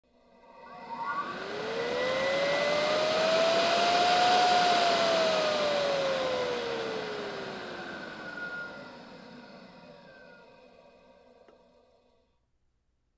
Electric Turbine Blower Whines and Runs at a High Speed with a Long Smooth Wind Down Take 2
Electric Turbine Blower Whines and Runs at a High Speed with a Long Smooth Wind Down Take 2 · The Hollywood Edge Sound Effects Library

High Tech Sound Effects, Vol. 4